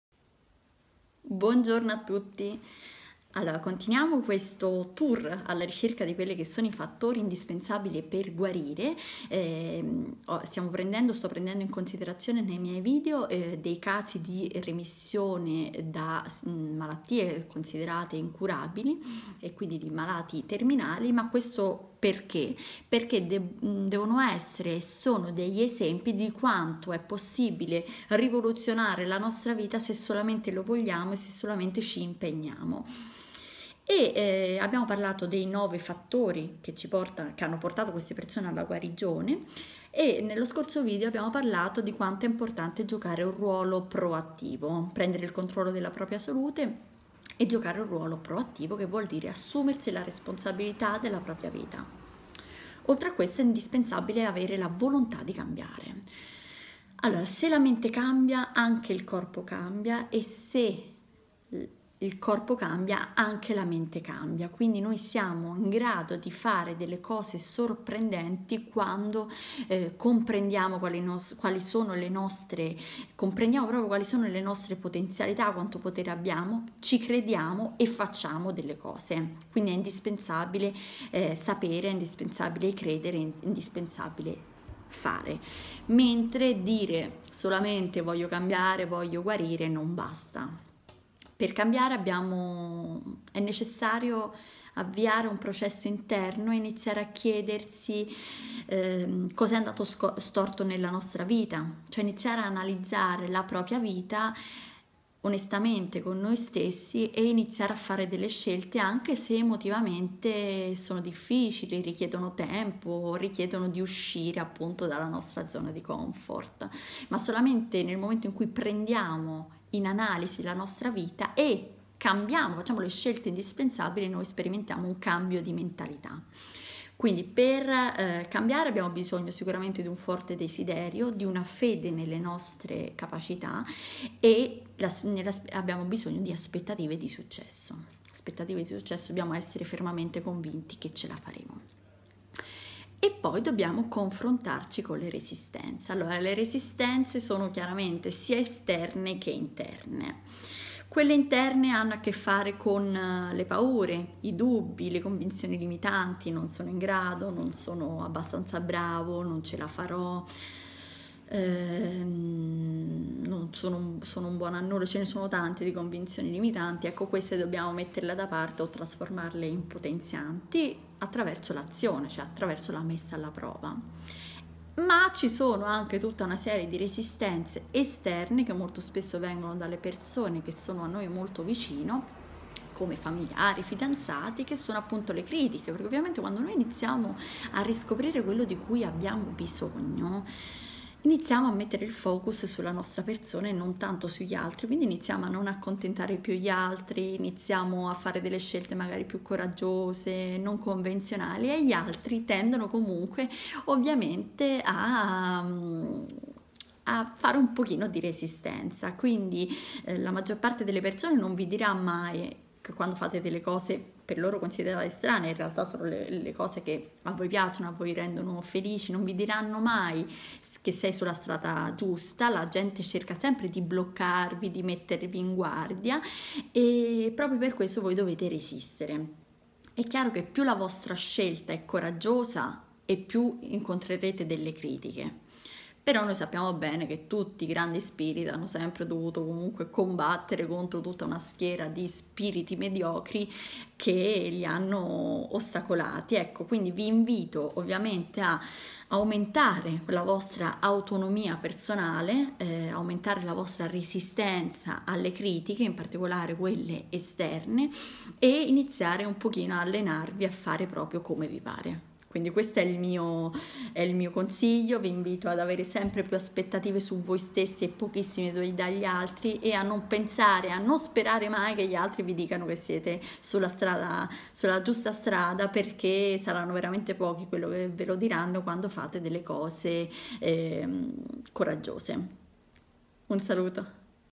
Guarigioni Straordinarie: Il Coraggio di CAMBIARE - Avere la Forza di Fare Scelte Radicali
✔️Dire "voglio cambiare" purtroppo non basta.
✔️Per guarire occorre il CORAGGIO di farsi le giuste domande e  saper ascoltare le risposte.
✔️Tutto questo significa analizzare la propria vita e fare scelte radicali, confrontandosi con le resistenze interne ma anche esterne.

NB: IL CONTENUTO VIDEO NON INTENDE SOSTITUIRE L'INTERVENTO INDIVIDUALE DI PROFESSIONISTI MA HA COME UNICO OBIETTIVO QUELLO DI CONDIVIDERE E DIVULGARE CONOSCENZE SULL'ARGOMENTO NELL'OTTICA DI PROMUOVERE IL BENESSERE DELL'INDIVIDUO.

Hai costantemente la sensazione di non essere all'altezza?